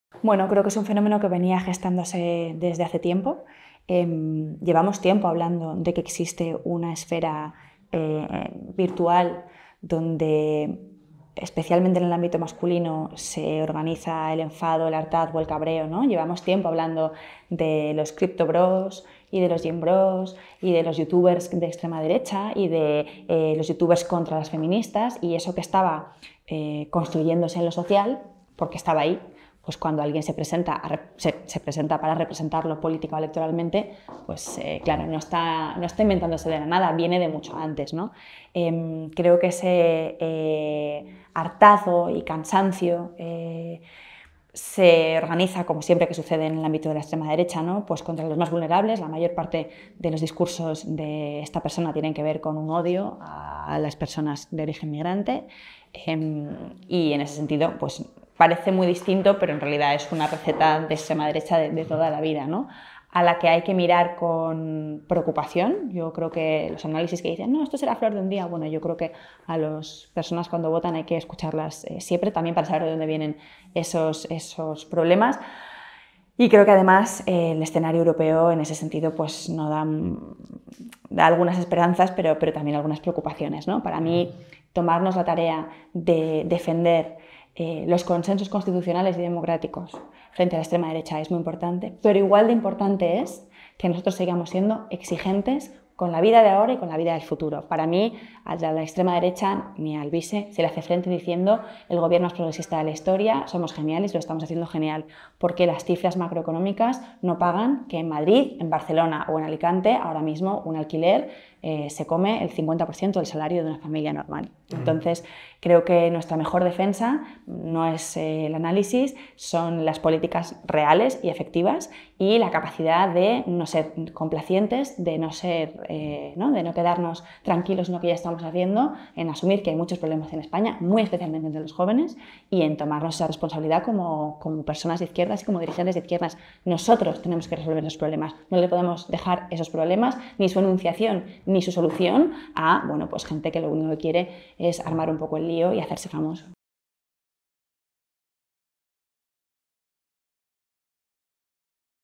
"Parece distinto, pero Alvise es en realidad una postura de extrema derecha de toda la vida"
infoLibre entrevista a la portavoz de Más Madrid en el Ayuntamiento de Madrid, Rita Maestre.